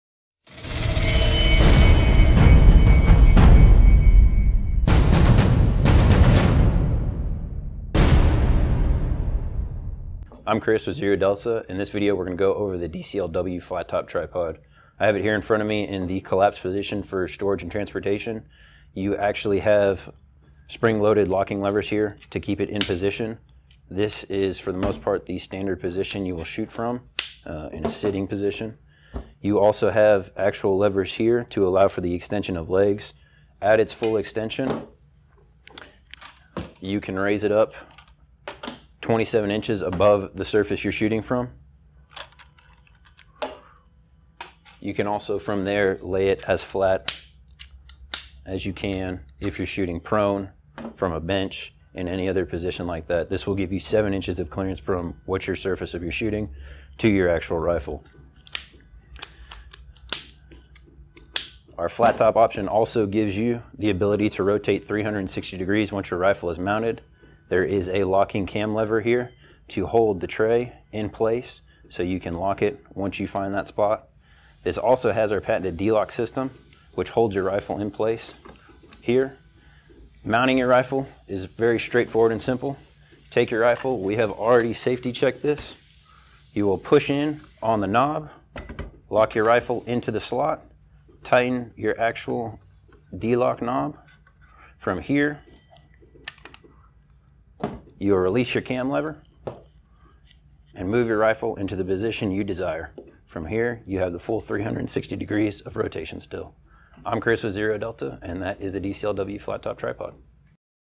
ZRODelta - DCLW Flattop Tripod
In this video, Chris will be showing you how to operate the ZRODelta DCLW Flattop Tripod.